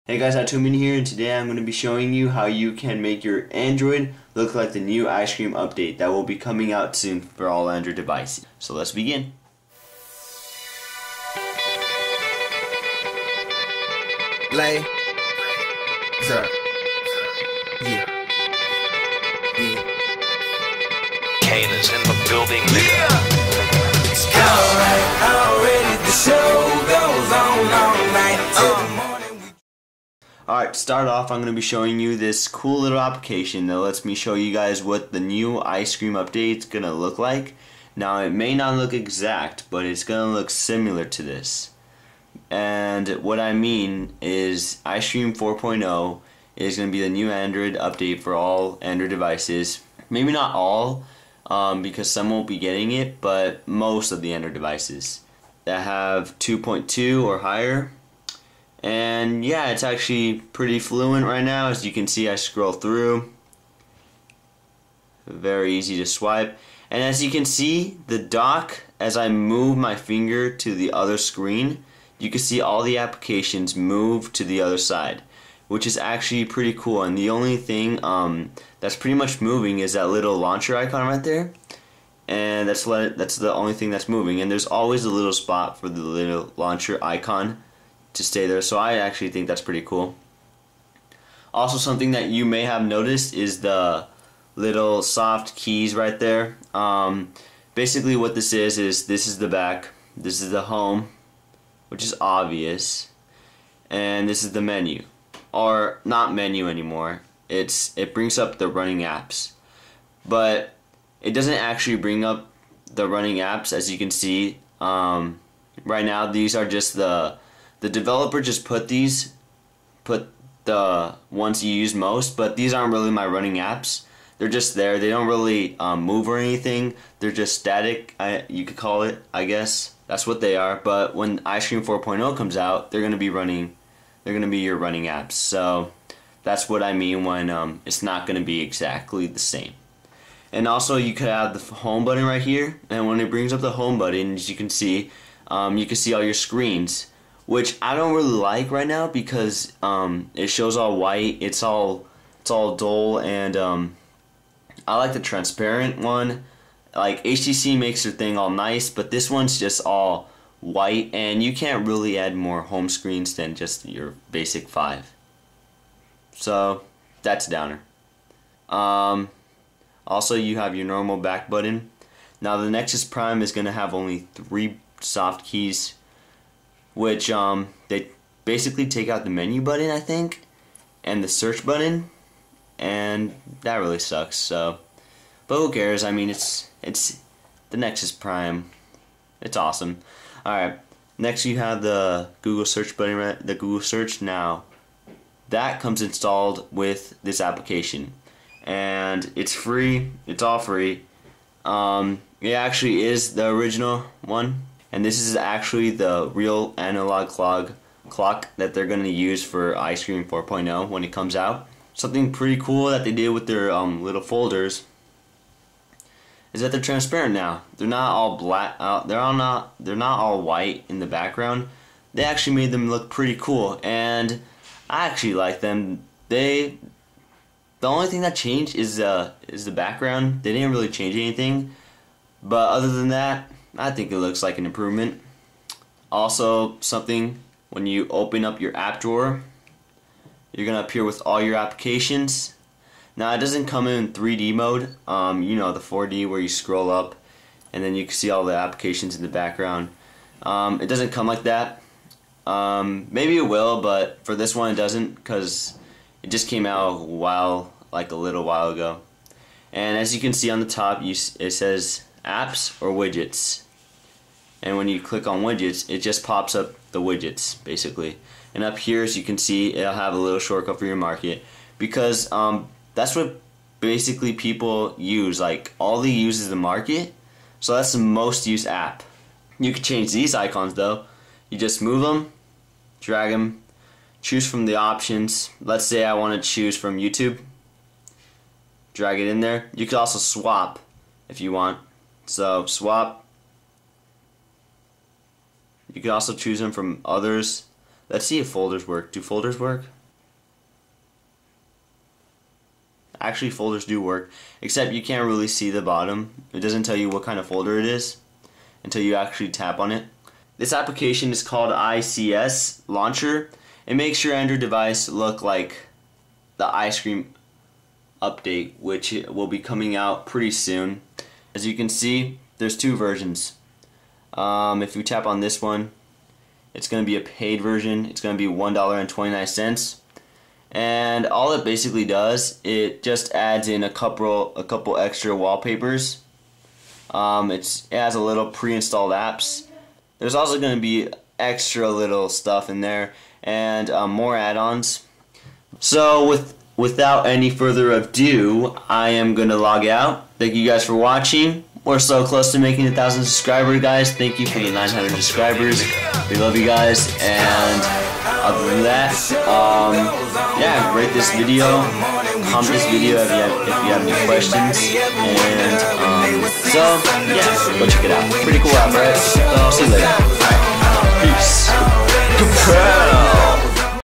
our How to get Ice Cream Launcher on yandroid device! No Root needed!
This application looks similar to the new Ice cream 4.0 update!

Facebook.com/howtomen